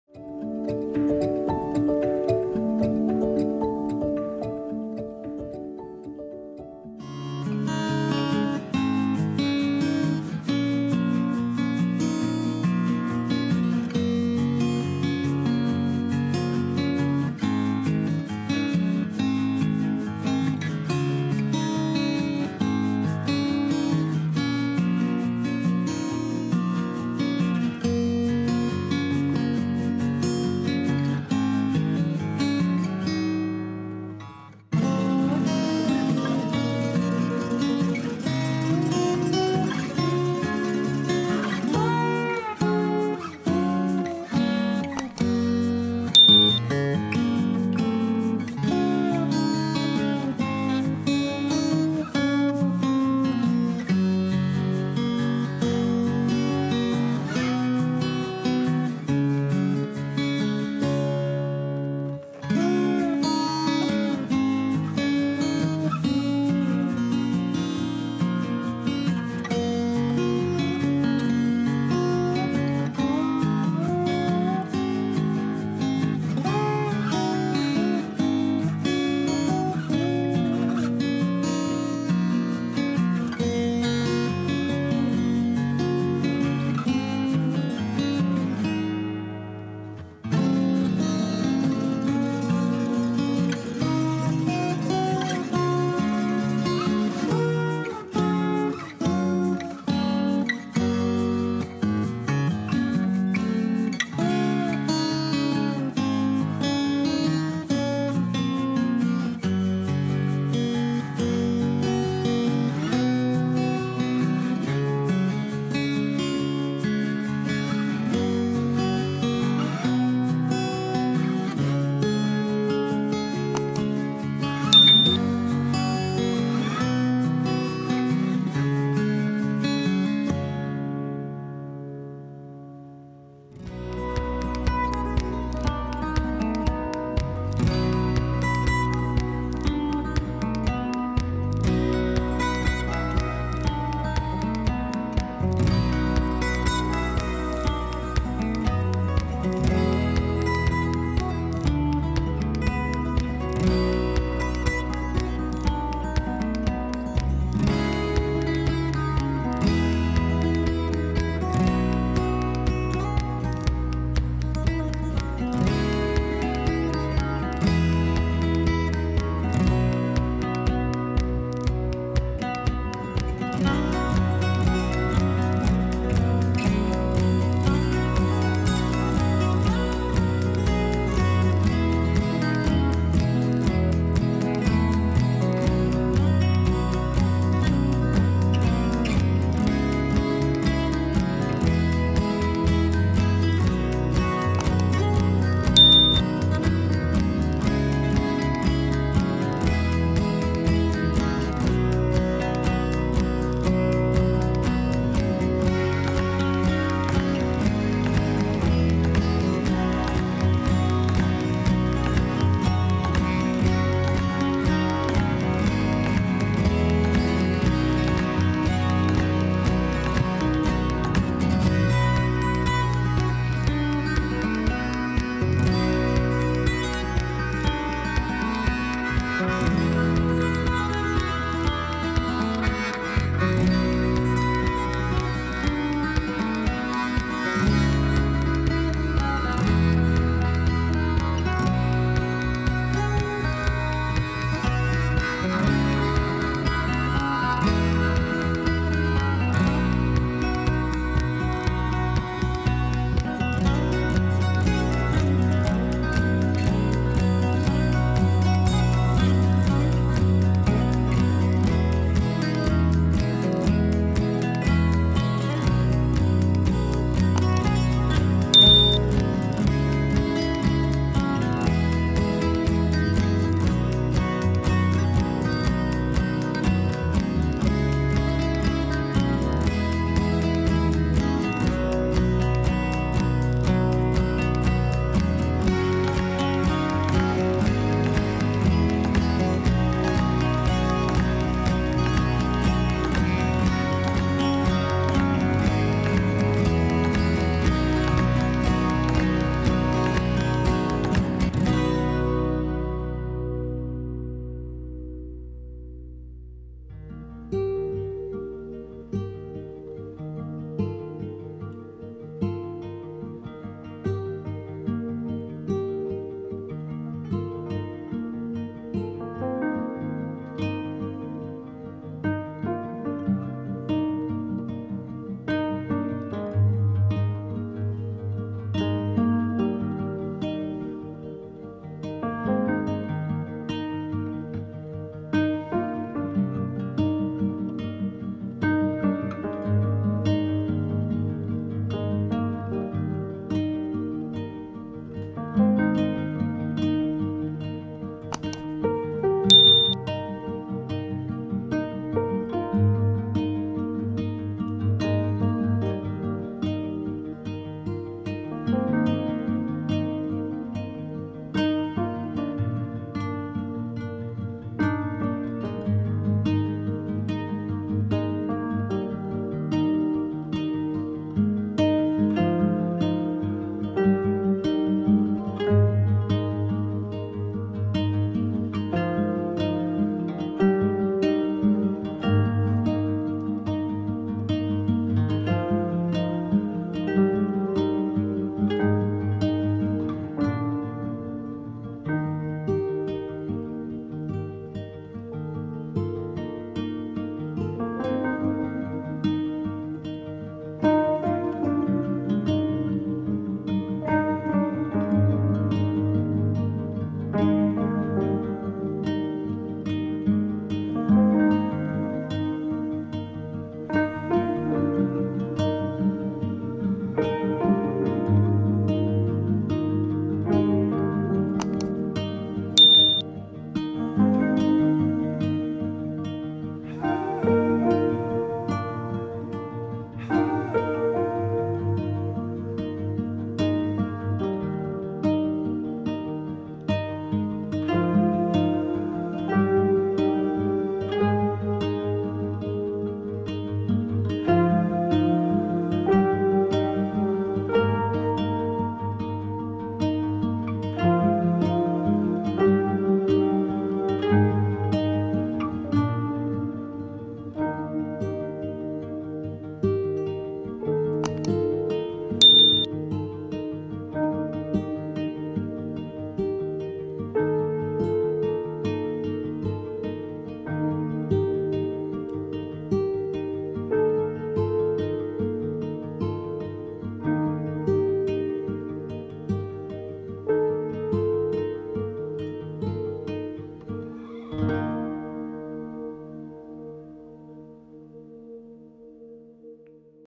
Recycling Plastic Bottles into Elephants-Shapes Planter Pots for Small Garden | Hanging Garden
0:00 | Intro
0:07 | Recycling Plastic Bottles into Elephants-Shapes Planter Pots for Small Garden | Hanging Garden
6:59 | Relax with Garden
Cement Ideas, Make Beautiful Flower Pots from Plastic Bottles and Cement For Your Garden